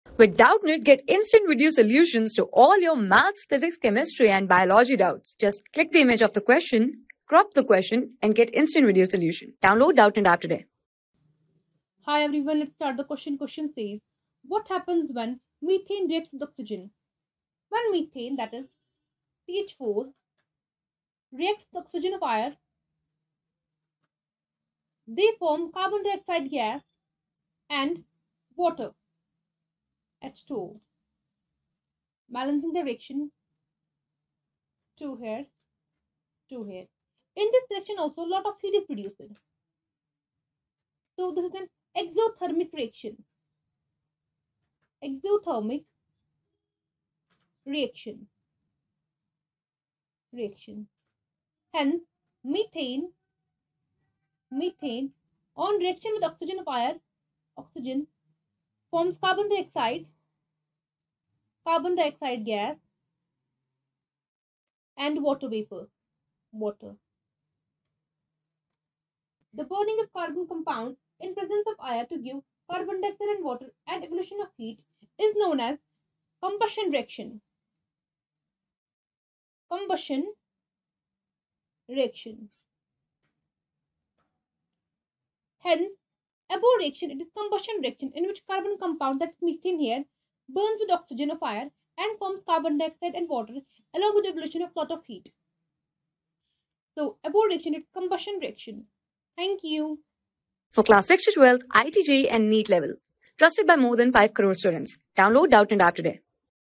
What happens when methane reacts with oxygen? | 8 | CARBON AND ITS COMPOUNDS | CHEMISTRY | ICSE...
What happens when methane reacts with oxygen?
Class: 8
Subject: CHEMISTRY
Chapter: CARBON AND ITS COMPOUNDS
Board:ICSE

👉 Have Any Query? Ask Us.